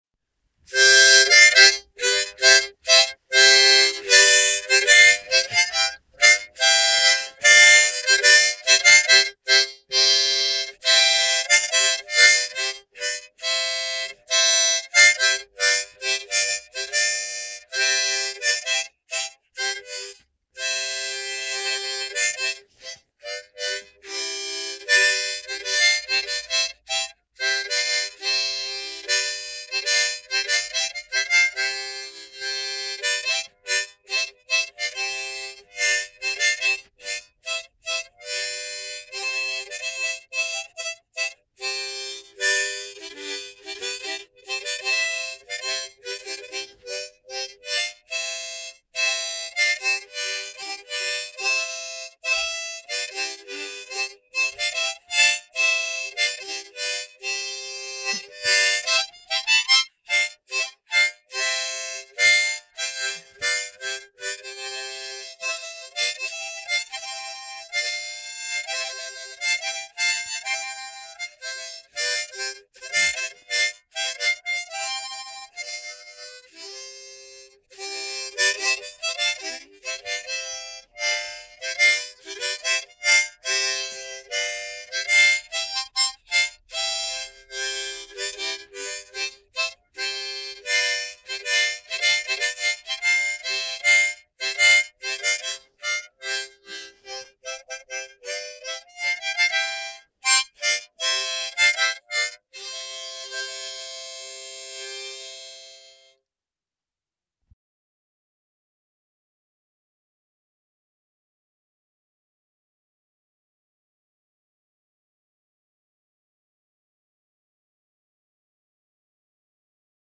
A rhapsody in C for mouth harp
Improvisation for the C harp (here a "Hohner Golden Melody No. 542")
Illustration in Word Paint by Hopesomewhere.